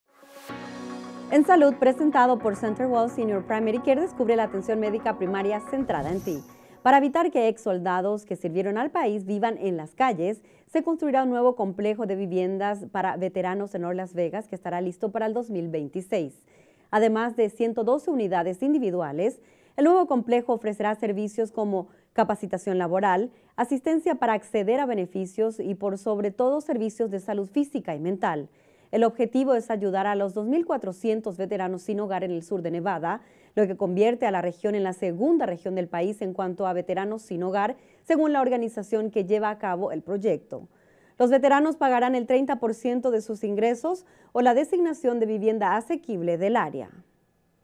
Salud: Cuidados de salud mental y física además de empleos para veteranos
Construirán viviendas asequibles para veteranos con asistencia integral en North Las Vegas.